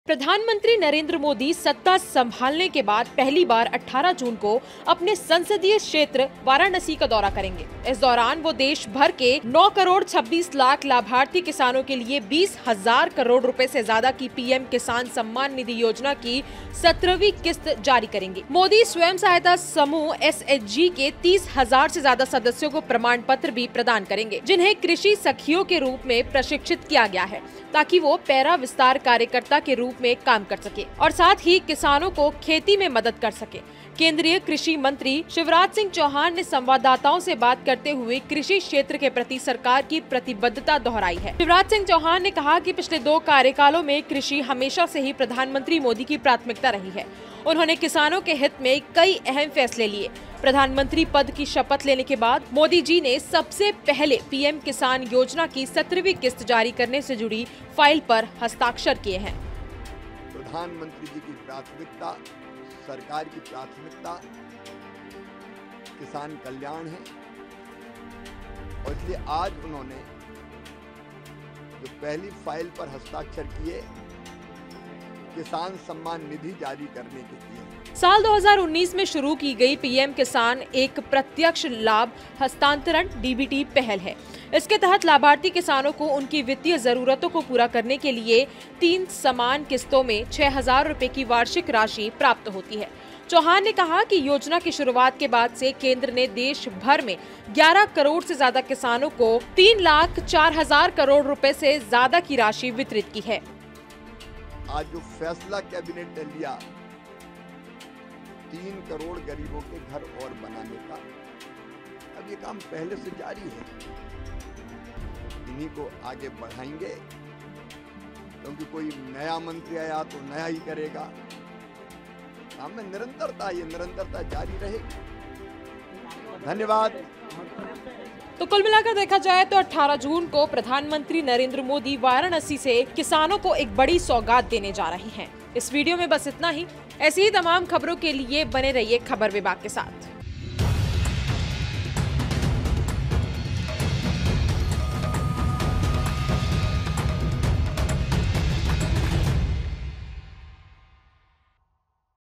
18 जून को जारी होगी PM Kisaan Samman Nidhi Yojana 17th Kist, कृषि मंत्री Shivraj Singh का ऐलान
18 जून को जारी होगी PM Kisaan Samman Nidhi Yojana 17th Kist, कृषि मंत्री Shivraj Singh का ऐलान

IBC24 के लोकसभा चुनाव EXIT POLL Survey में हिस्सा लें